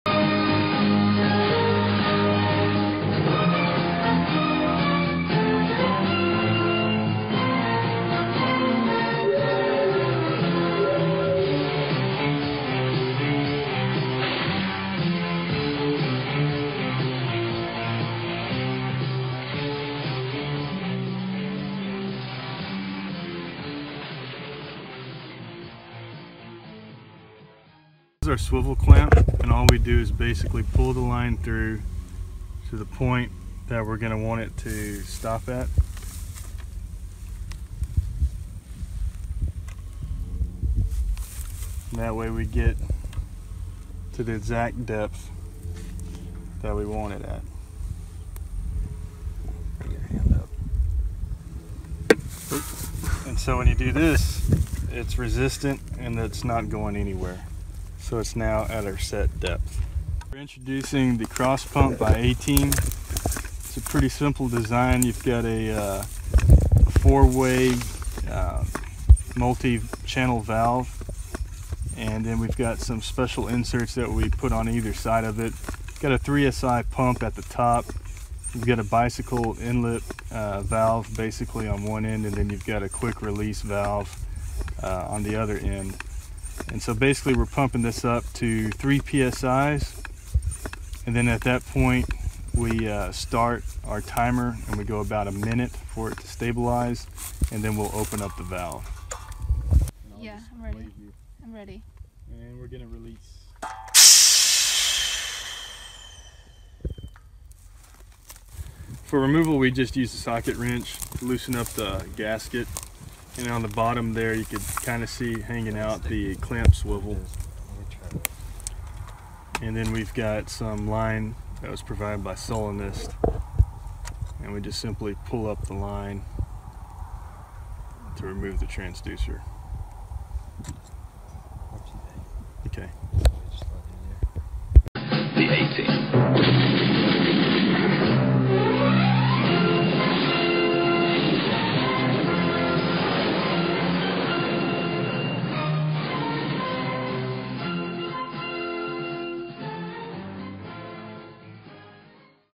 pneumatic slug test device
pneumatic slug test procedure performed during TAMU GEOLOGY 610 course.